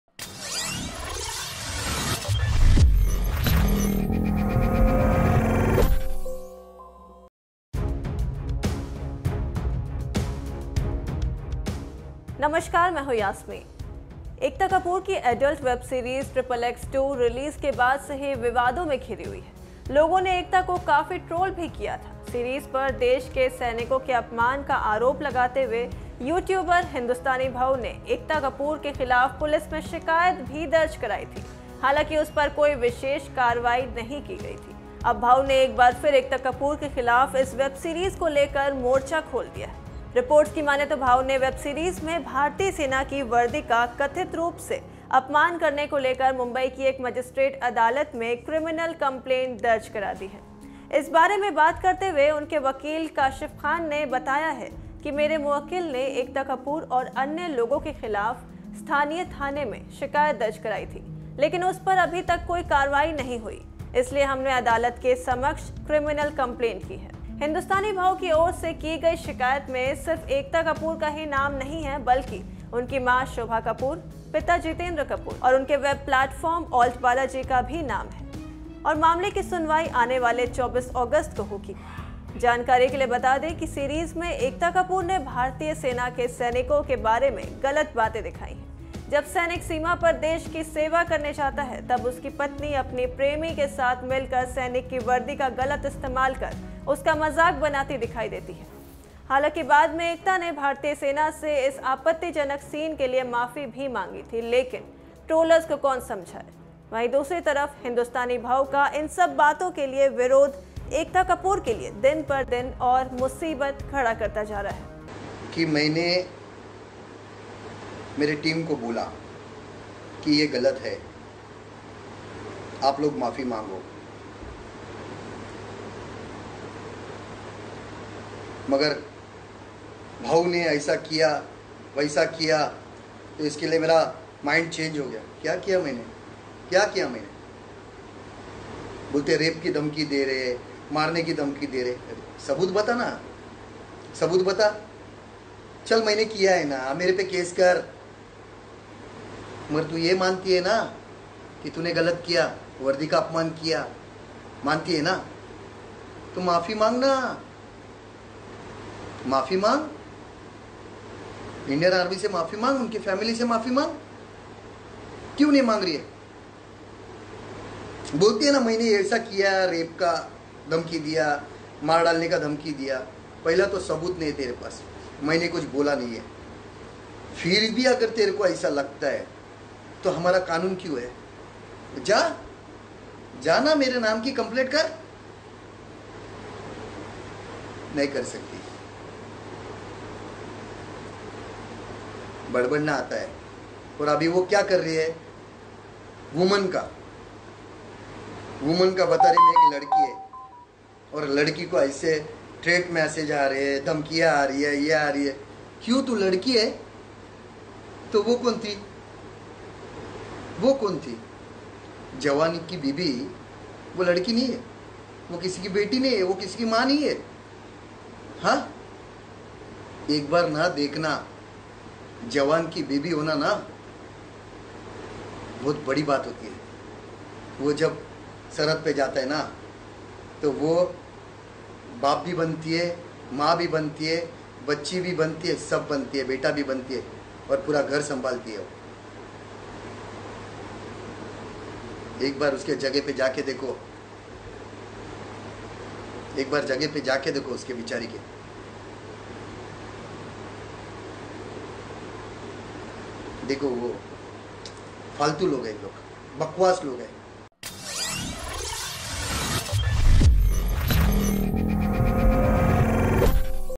Ekta Kapoor की फिर बढ़ी मुश्किलें, Hindustani Bhau ने खोला मोर्चा
Ekta Kapoor की वेब सीरीज ट्रिपल एक्स 2 को लेकर शुरूआत से ही काफी विवाद हो रहा है…यूट्यूबर हिंदूस्तानी भाऊ ने एकता कपूर के खिलाफ पुलिस में शिकायत दर्ज कराने के बाद एक बार मोर्चा खोल दिया है.. जानिए मामला